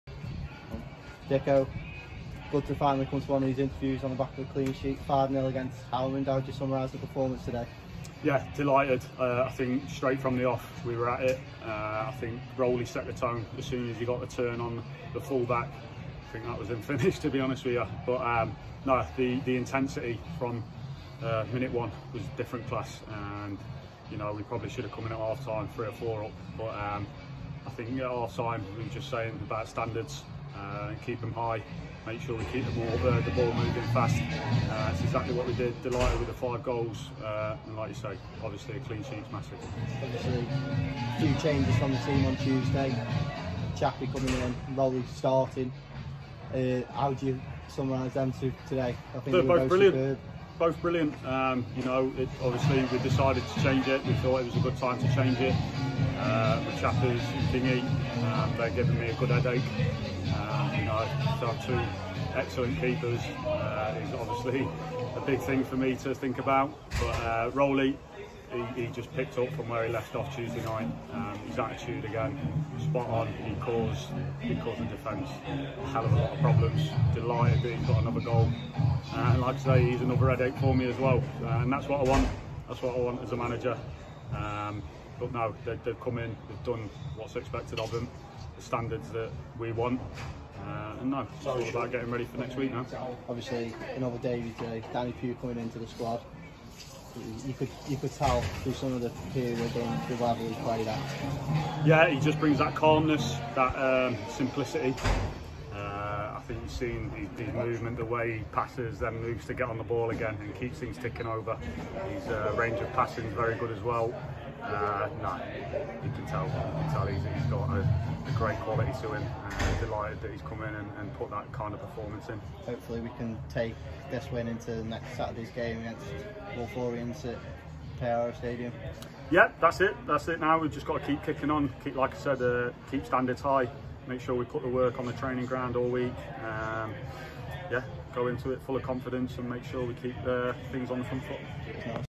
Carl Dickinson | Post Match Interview vs Haughmond
The gaffer speaks to us following yesterday’s 5-0 win over haughmond